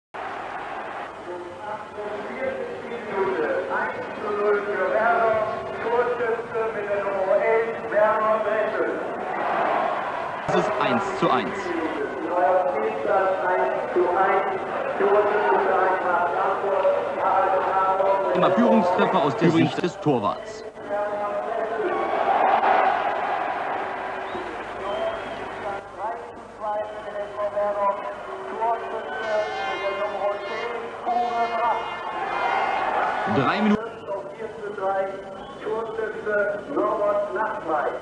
SV Werder Stadionsprecher Spezi Mayr.
Die ganz alten Hasen werden sich vielleicht noch erinnern: Bremer  Stadionsprecher und Diskothekenlegende "Spezi" Karl-Heinz Mayr († 2008)"Spezi" Karl-Heinz Mayr war der Vorgänger von Christian Günther.Er war von 1976 bis 1981 Werders Stadionsprecher im Weser-Stadion.
Ich habe das mal schnell zusammen geschnitten.